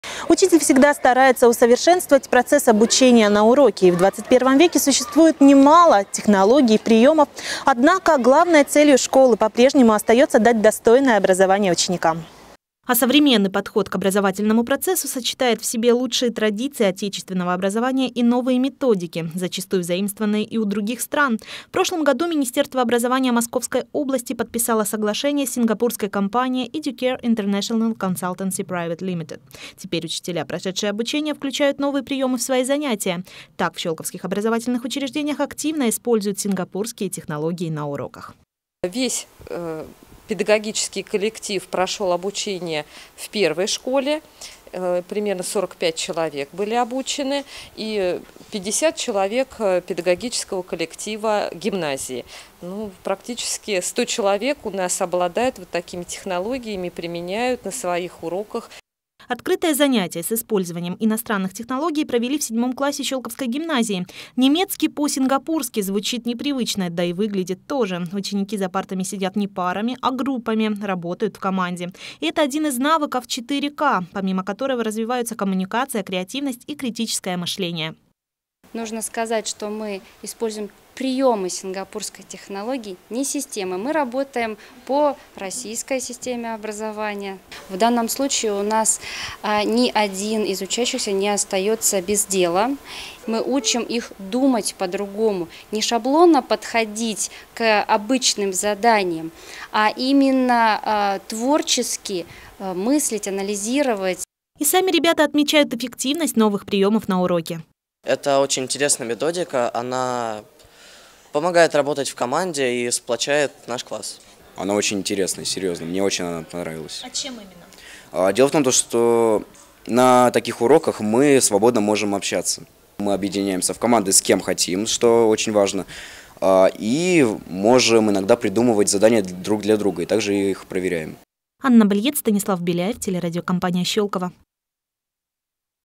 Сингапурские технологии в Щёлковской гимназии
В Щёлковской гимназии провели открытый урок немецкого языка по сингапурским технологиям. В чем суть методики и какие у неё преимущества, расскажет наш корреспондент.